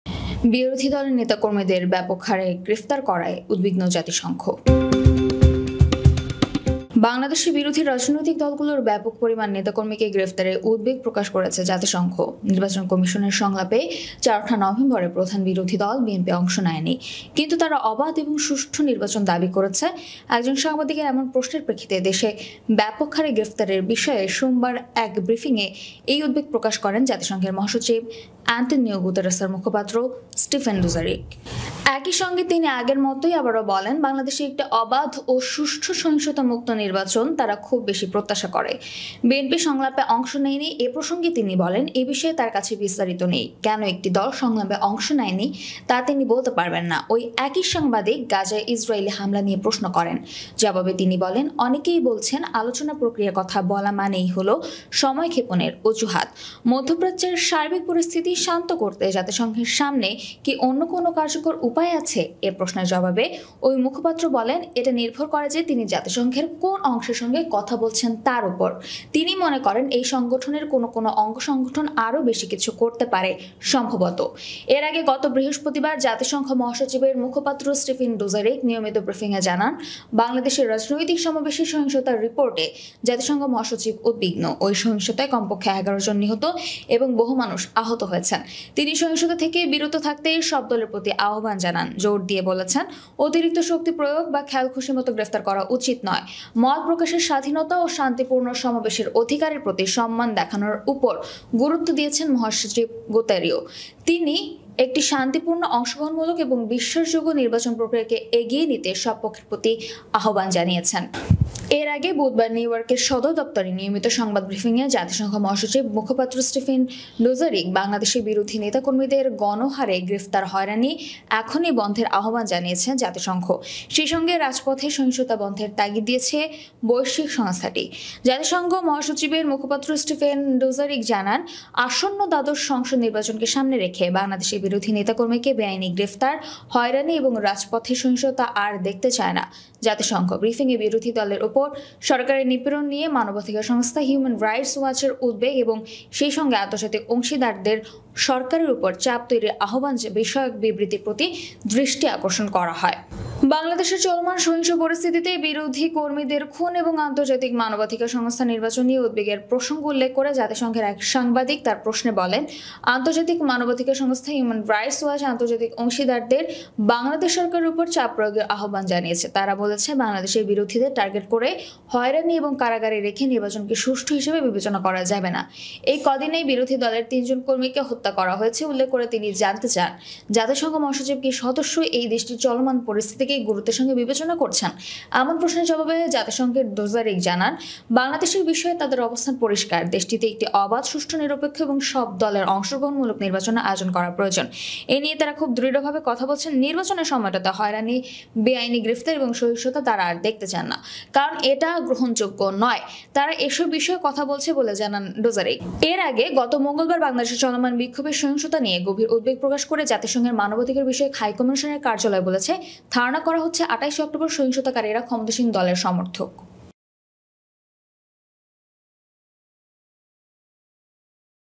বিরোধী দলের নেতাকর্মীদের ব্যাপকহারে গ্রে' প্তা*র করায় উদ্বিগ্ন জাতিসংঘ | UN | BD Police | BD News
বাংলাদেশে বিরোধী রাজনৈতিক দলগুলোর ব্যাপক পরিমাণ নেতাকর্মীকে গ্রেপ্তারে উদ্বেগ প্রকাশ করেছে জাতিসংঘ। নির্বাচন কমিশনের সংলাপে ৪ঠা নভেম্বরে প্রধান বিরোধী দল বিএনপি অংশ নেয়নি। কিন্তু তারা অবাধ ও সুষ্ঠু নির্বাচন দাবি করছে-  একজন সাংবাদিকের এমন প্রশ্নের প্রেক্ষিতে দেশে ব্যাপকহারে গ্রেপ্তারের বিষয়ে সোমবার এক ব্রিফিংয়ে এই উদ্বেগ প্রকাশ করেন জাতিসংঘ মহাসচিব অ্যান্তোনিও গুতেরাঁর মুখপাত্র স্টিফেন ডোজারিক। একই সঙ্গে তিনি আগের মতোই আবারও বলেন, বাংলাদেশে একটি অবাধ ও সুষ্ঠু, সহিংসতামুক্ত নির্বাচন তারা খুব বেশি প্রত্যাশা করে।

ধন্যবাদ..